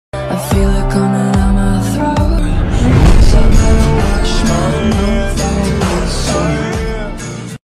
Guess I better wash my with soap meme ft me and my grandma (vent video)
Don’t get me wrong, I love my grandma to death, she’s a good person, she helped me when I was down, when I feel bad about myself she was there to tell me that I’m special, but she doesn’t understand that I’m not interested in romantic relationships. I mean I was interested when I was 15 but then I realized that I don’t need a man, I can be happy by myself, but my grandma thinks I just need some time to “think it over” but I already made up my mind, and I’m sticking with it.